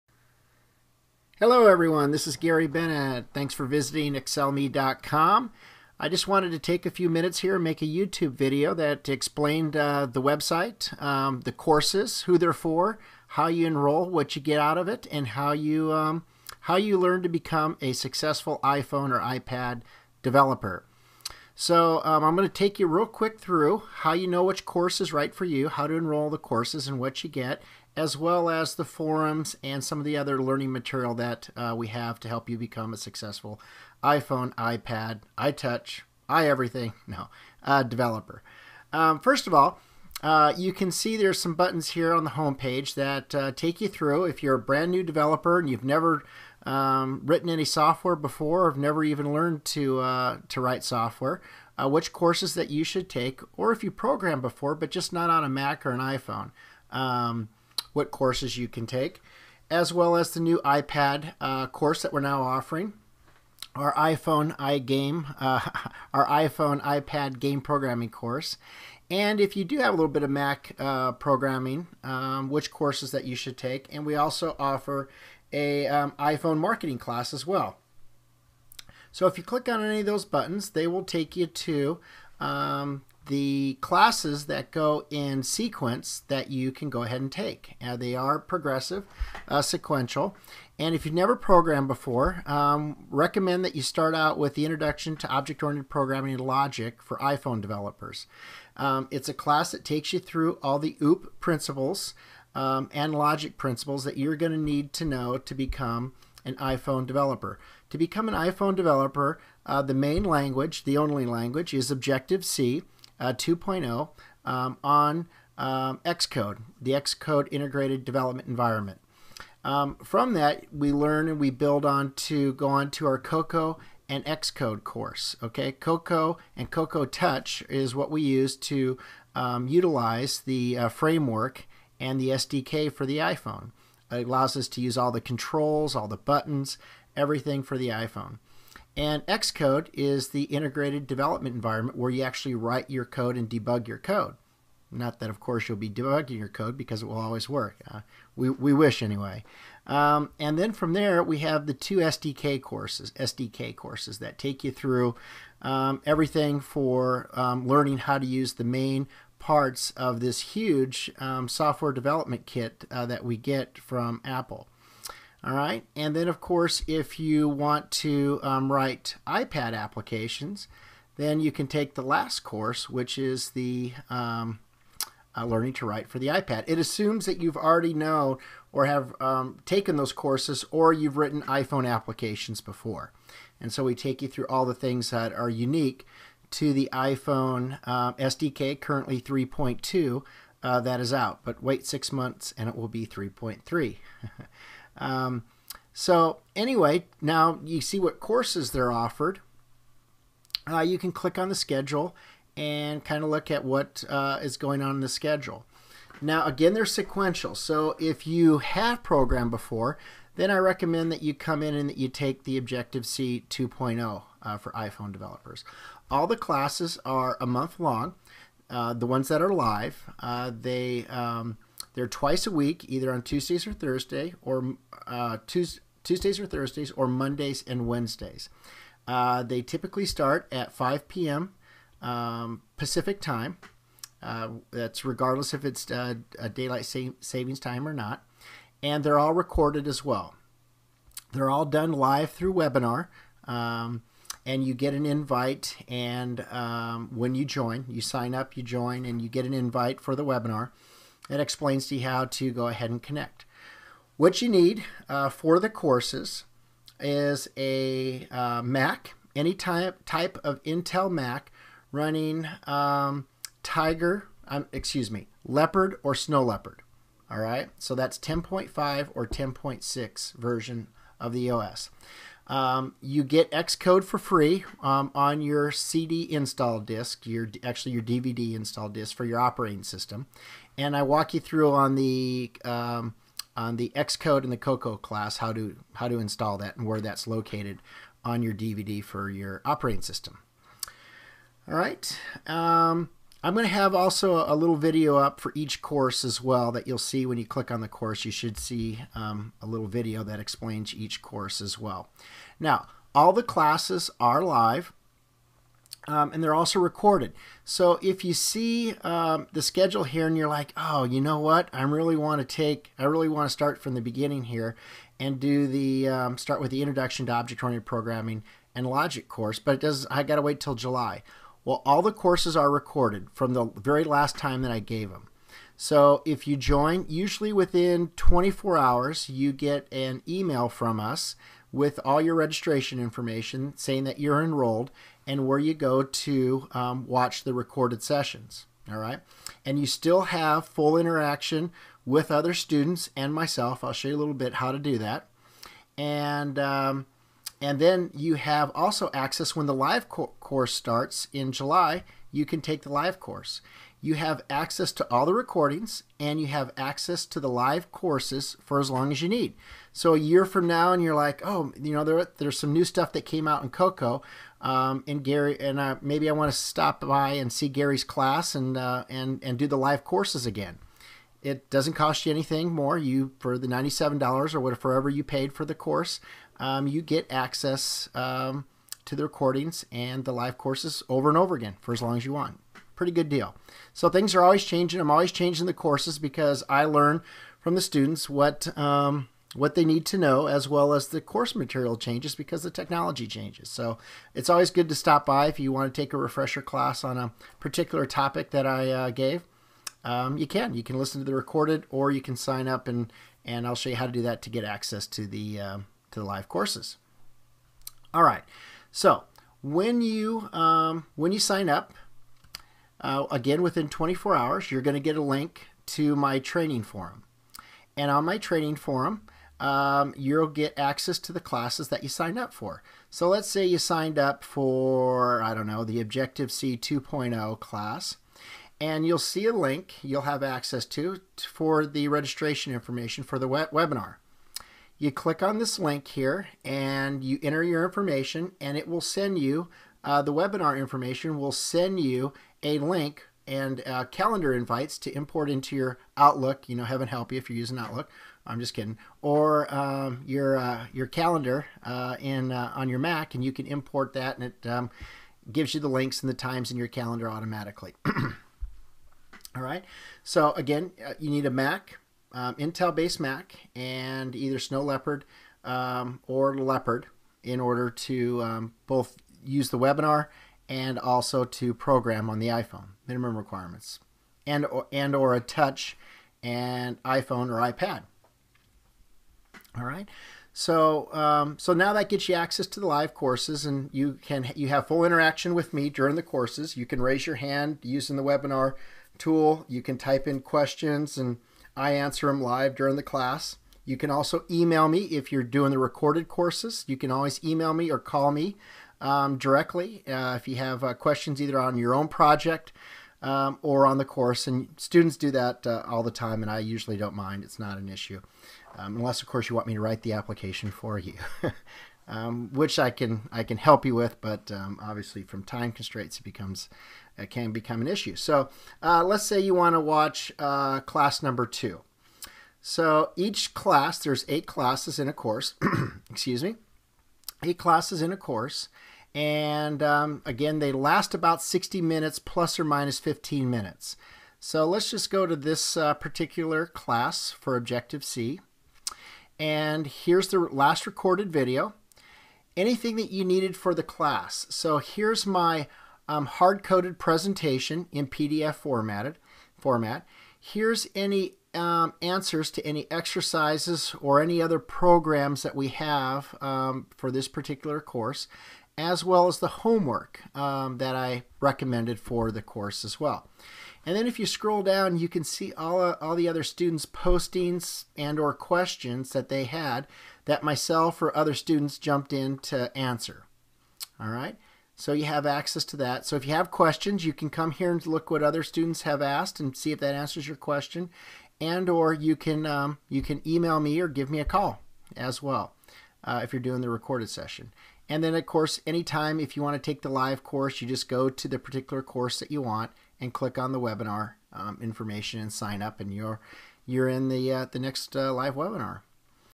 xcelMe.com - Introduction to iPhone and iPad Programming Courses Part 1 of 2
Frequently Asked Questions discussed about xcelMe.com iPhone and iPad courses. Gary Bennett discusses technology used, recordings and live webinars along with how to becoming a successful iPhone/iPad developer